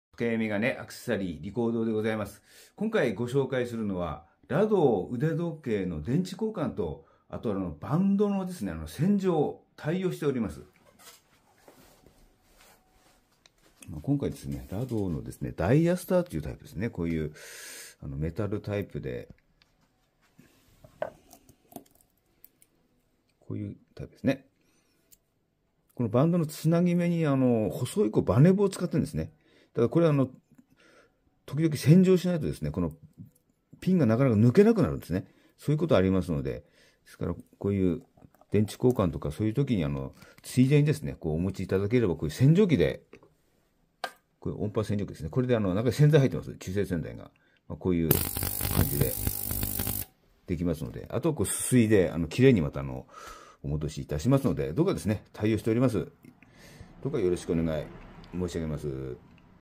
室蘭市 腕時計電池交換 ラドー腕時計の電池交換とバンドの洗浄 対応
りこう堂 〒051-0011 北海道室蘭市中央町2丁目4-14(旧ア－ケ－ド街)
☎️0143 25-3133☎️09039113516 お問い合わは お気軽に LINEで⇒ ホームページ ⇒